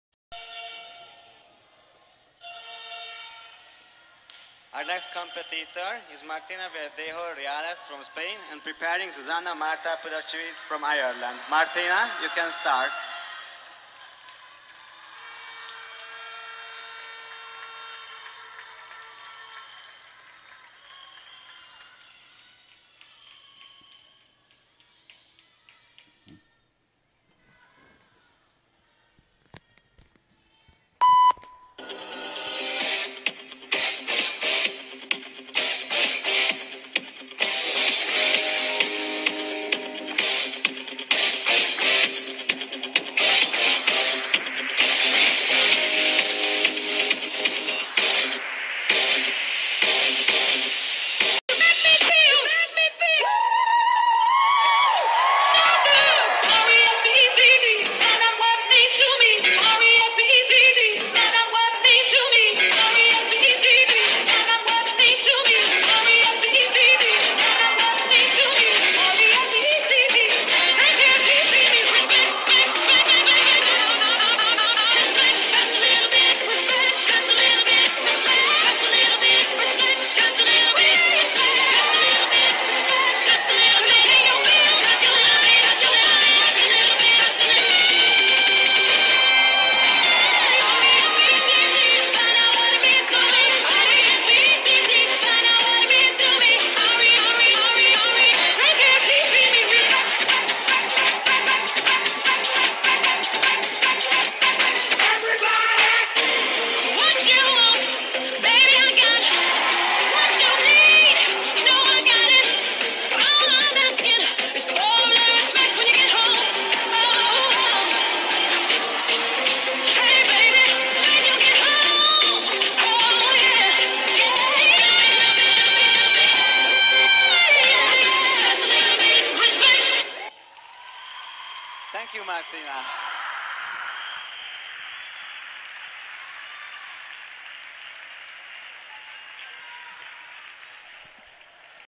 Martina Verdejo Fit Kid 2019
Campeona Regional, Campeona de España y Sub-Campeona de Europa.
Centro Abaeza